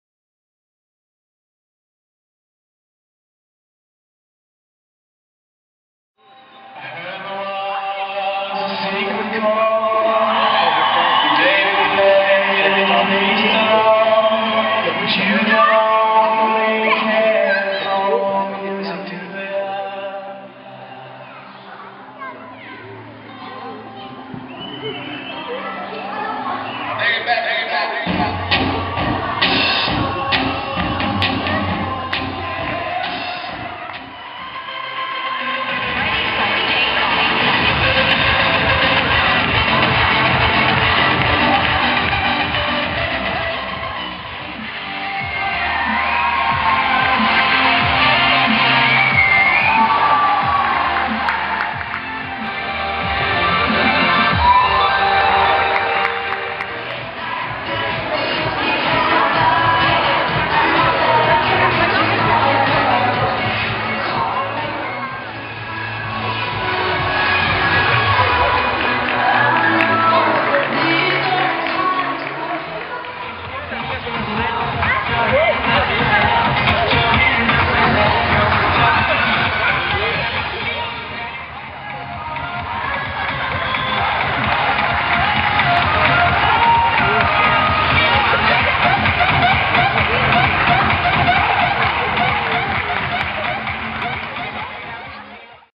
Turnsportens dag
Turnsportens dag og turnoppvisning fra Melbo Turnforening